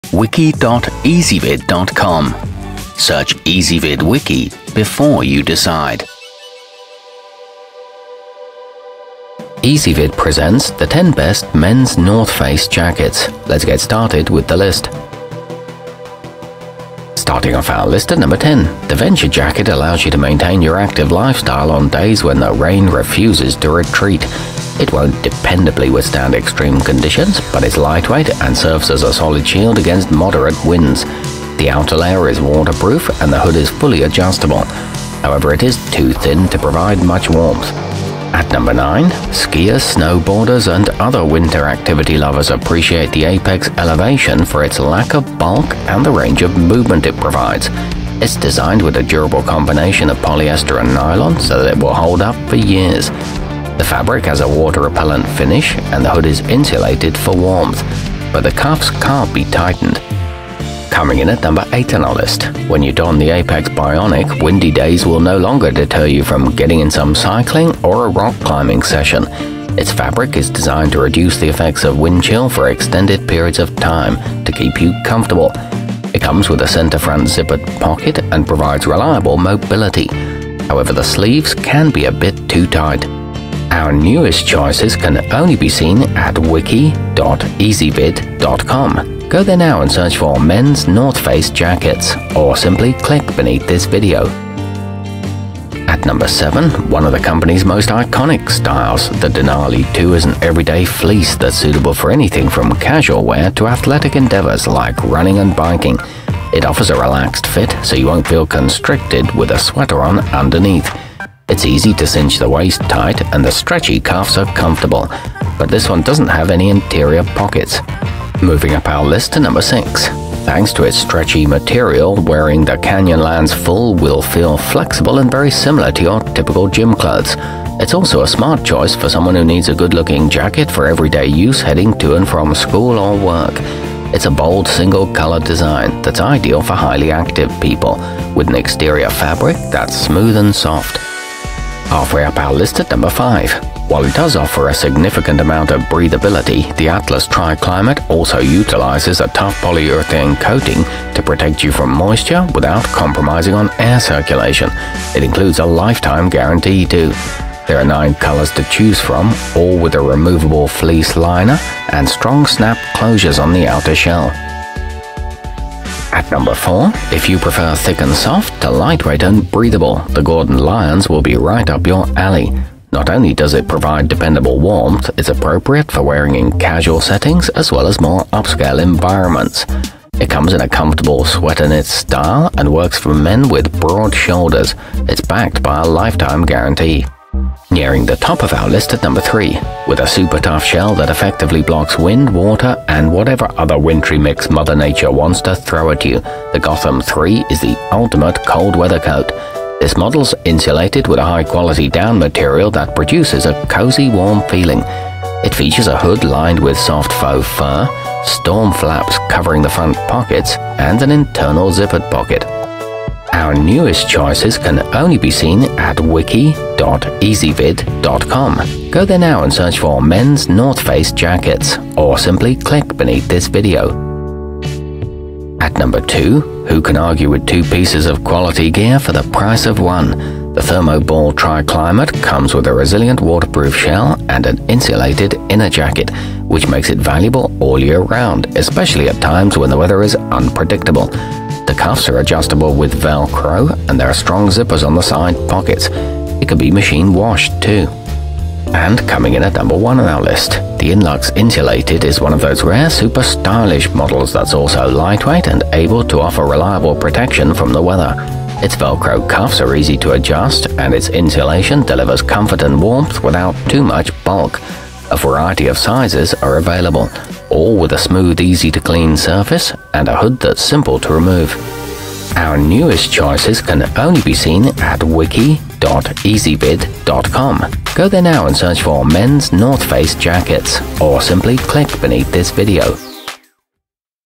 10 Best Men's North Face Jackets 2018
Our complete review, including our selection for the year's best men's north face jacket, is exclusively available on Ezvid Wiki.

Men's north face jackets included in this wiki include the denali 2, apex elevation, gordon lyons, gotham iii, venture jacket, atlas tri-climate, canyonlands full, thermoball triclimate, inlux insulated, and apex bionic.